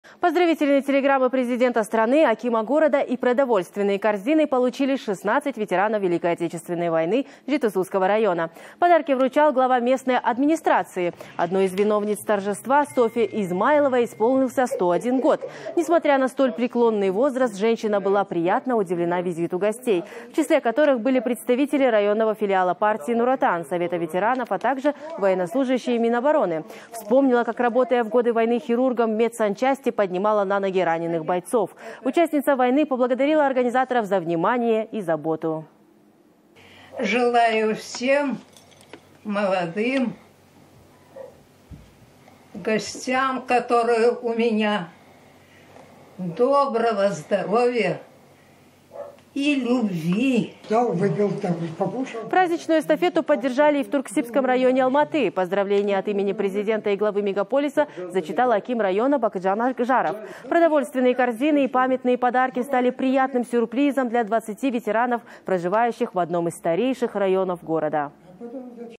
101-летнюю алматинку поздравили с предстоящим Днем Победы власти Жетысуского района (07.05.20)
Поздравительные телеграммы Президента страны, акима города и продовольственные корзины получили 16 ветеранов Великой Отечественной войны Жетысуского района. Подарки вручал глава местной администрации Максат Кикимов, передает корреспондент Almaty.tv.

Одной из виновниц торжества - Софе Измайловой исполнился 101 год. Женщина была приятна удивлена визиту гостей в числе которых были представители районного филиала партии "Nur Otan", совета ветеранов, а также военнослужащие Минобороны. Вспомнила ветеран, как, работая в годы войны хирургом, поднимала на ноги раненых бойцов. Участница ВОВ поблагодарила организаторов за внимание и заботу.

«Желаю всем молодым гостям, которые у меня, доброго здоровья и любви», - сказала ветеран Великой Отечественной войны Софа Измайлова.

Праздничную эстафету поддержали и в Турксибском районе Алматы. Поздравления от имени Президента и главы мегаполиса зачитал аким района Бахытжан Акжаров. Продовольственные корзины и памятные подарки стали приятным сюрпризом для 20 ветеранов, проживающих в одном из старейших районов города.